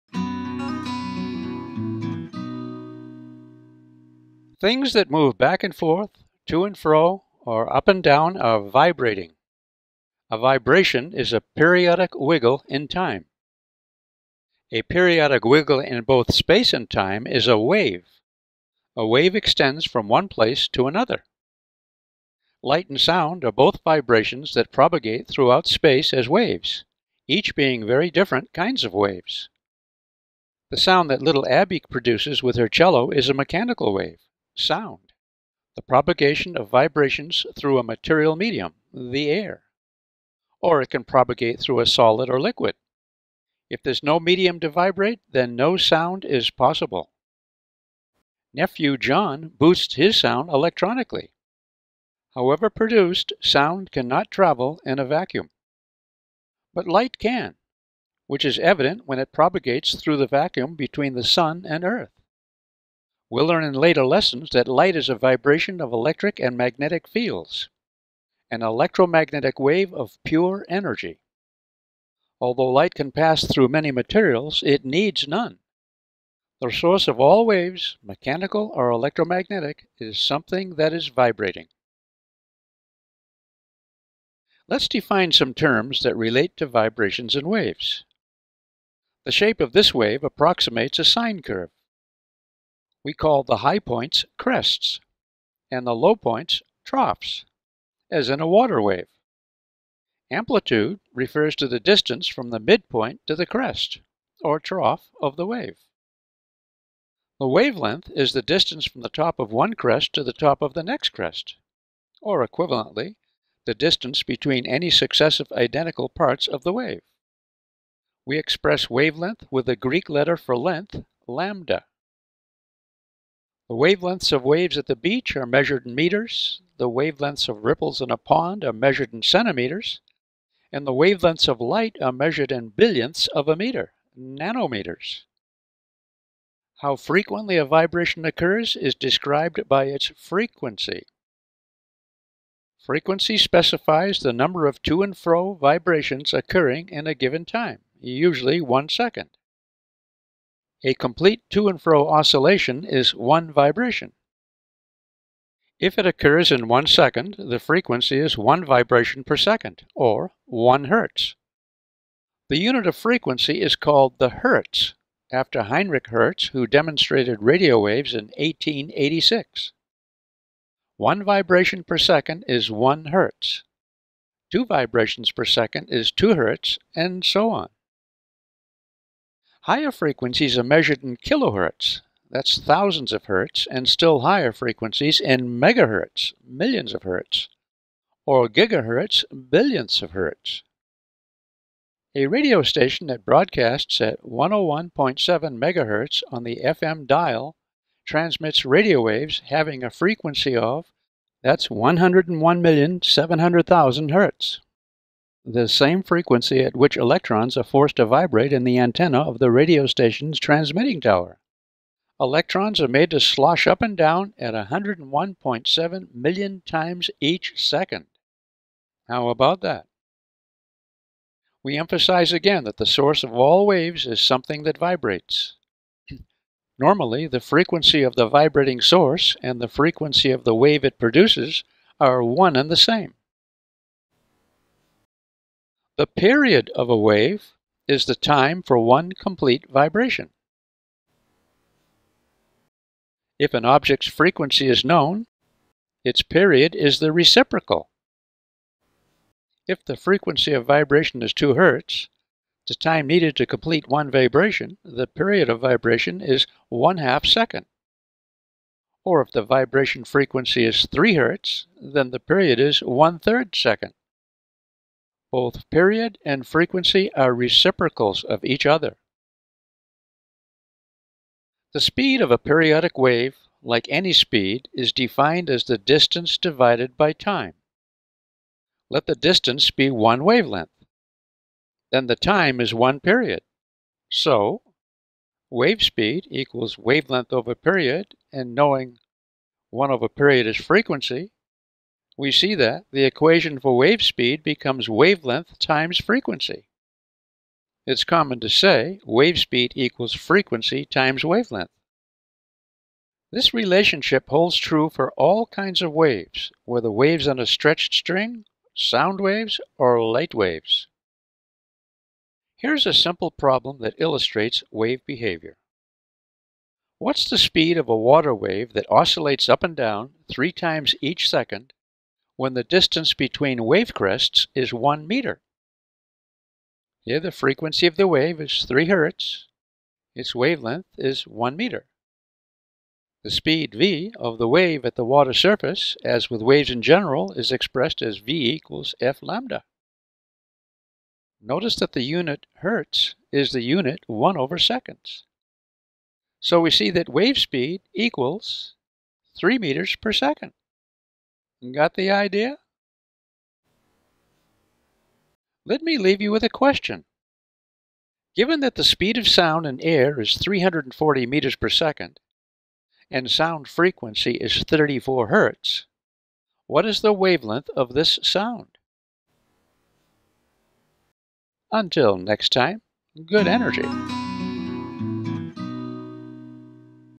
Hewitt-Drew-it! PHYSICS 82. Good Vibrations and Waves
Vibrations, the waves they produce, and wave speed, are described and explained.